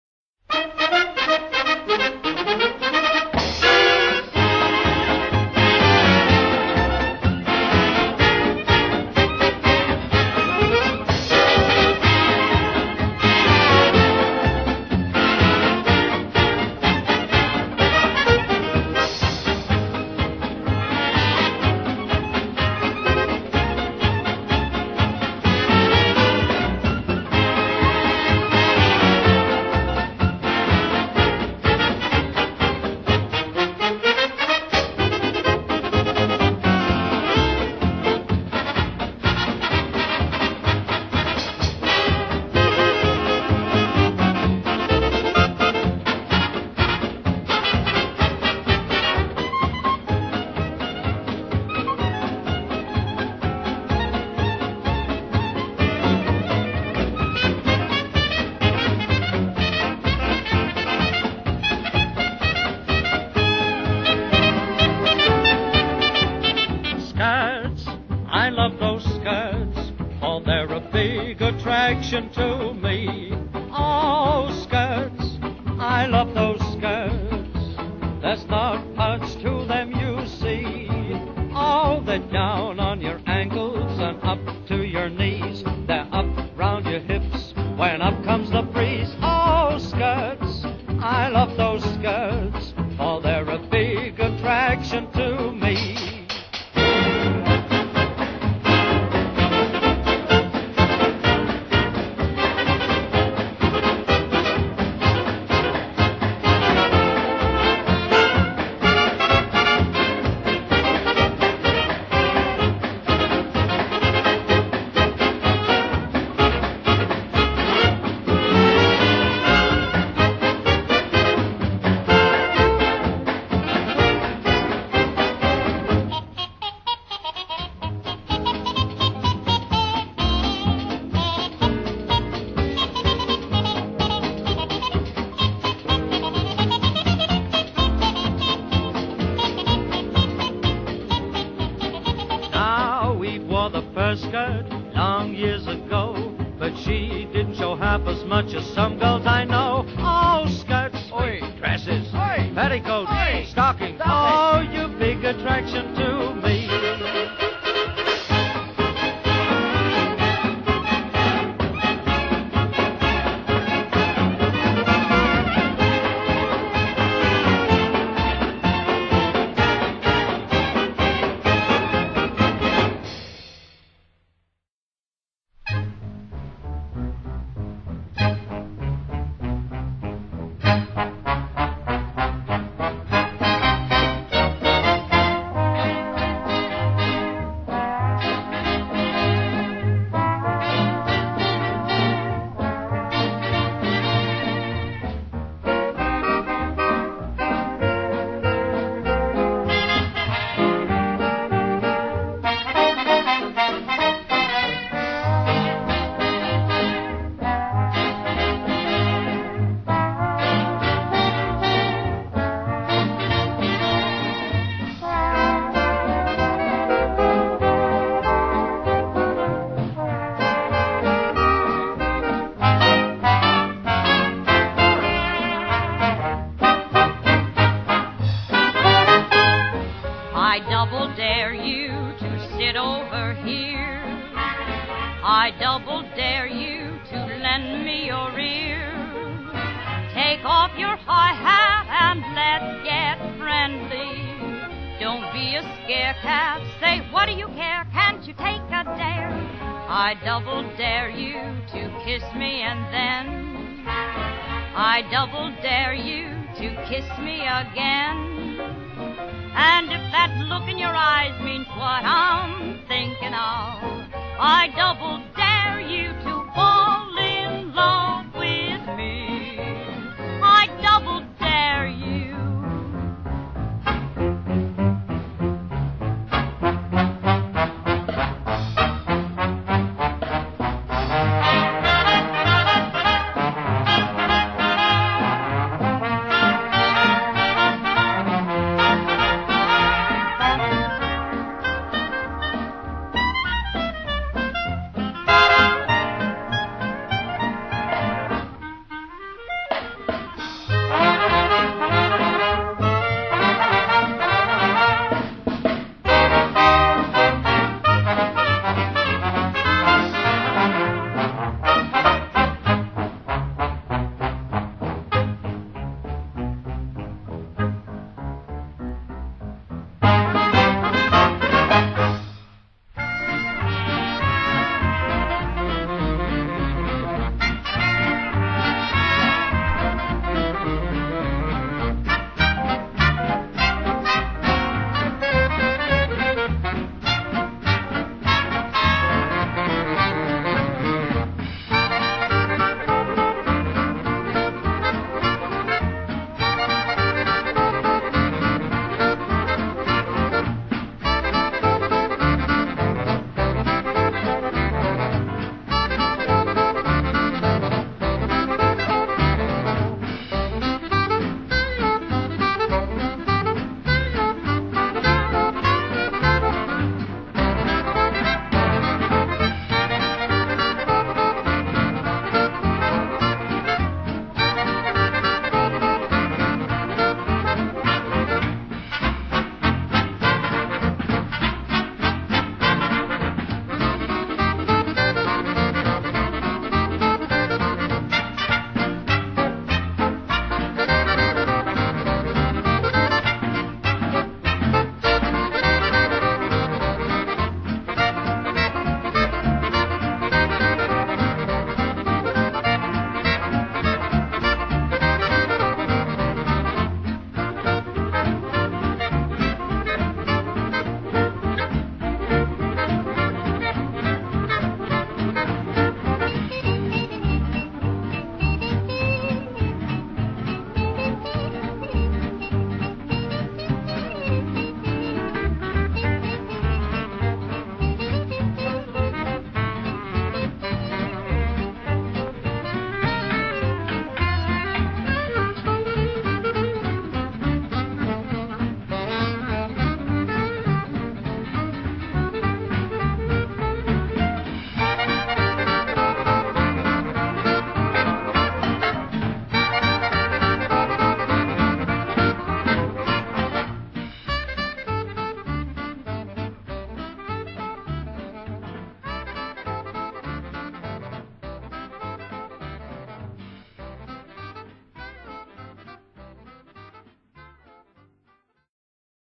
9.5mm Charlie Chaplin Film
This old 9.5 mm cine film was one of the first to be uploaded using our new custom built 9.5mm telecine machine. We added appropriate music too!